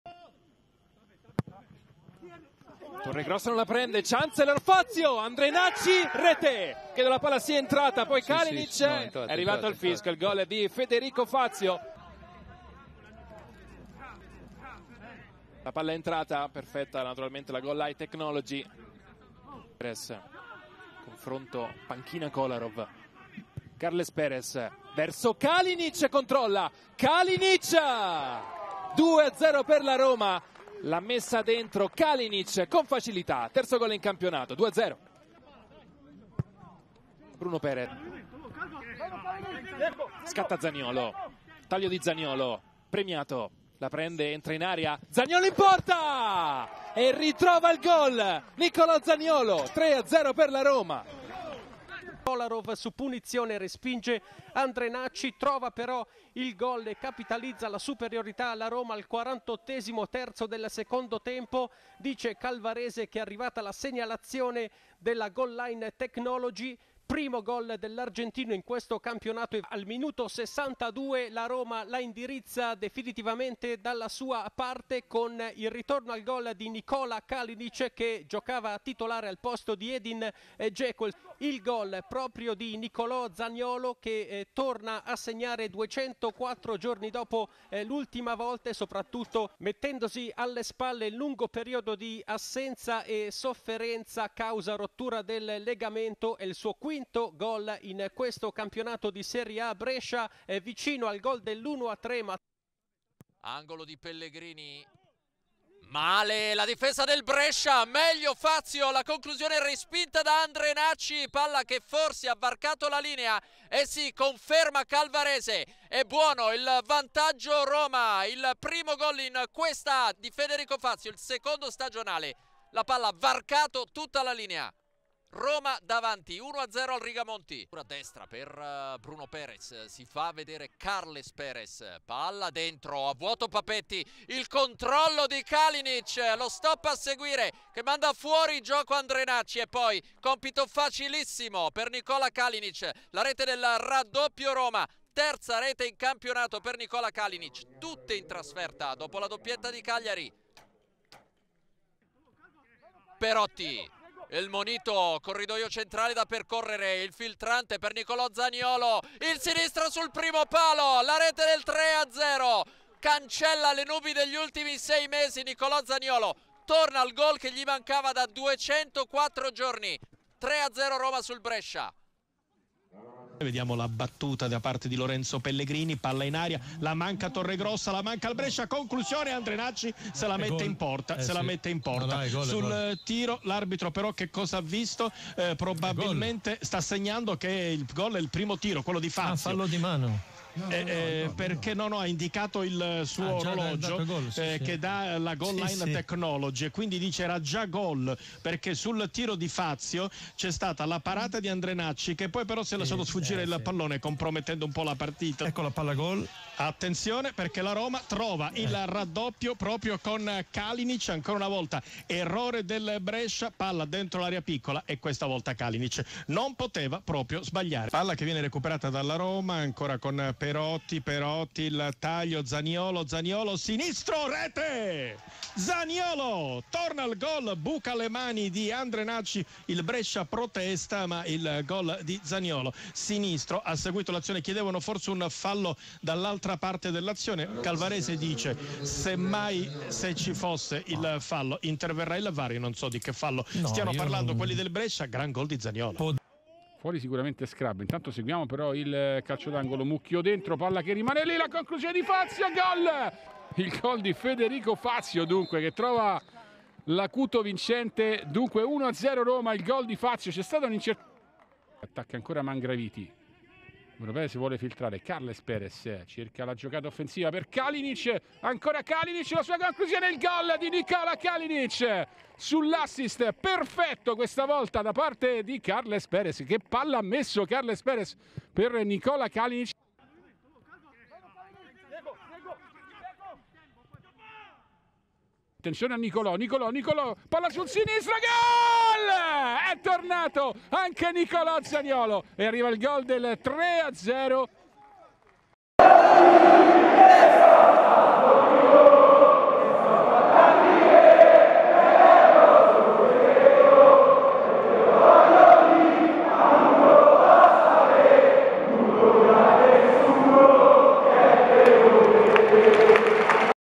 Brescia 0-3 Roma Highlights Serie A TIM, SKY, RomaTV, SportMediaset
Gli highlights di Brescia 0-3 Roma con il commento di Serie A TIM, SKY, RomaTV, SportMediaset

BRESCIA (4-3-1-2): Joronen; Sabelli, Chancellor, Papetti, Mangraviti (42' st Semprini); Bjarnason (13' st Donnarumma), Tonali, Ndoj (1' st Dessena); Spalek (36' st Ghezzi); Torregrossa, Skrabb (1' st Zmrahl). A disposizione: Andrenacci, Gastaldello, Martella, Viviani, Ayé. Allenatore: Lopez

ROMA (3-4-2-1): Mirante; Mancini (37' st Spinazzola), Fazio, Ibanez; Bruno Peres, Diawara, Veretout (22' st Villar), Kolarov; Carles Perez (22' st Perotti), Pellegrini (22' st Zaniolo); Kalinic (34' st Dzeko). A disposizione: Pau Lopez, Fuzato, Çetin, Zappacosta, Pastore, Kluivert. Allenatore: Fonseca

ARBITRO: Calvarese di Teramo

MARCATORI: 3' st Fazio, 17' st Kalinic, 30' st Zaniolo

NOTE: Ammoniti: Peres, Perotti (R). Recupero: 1' pt e 5' st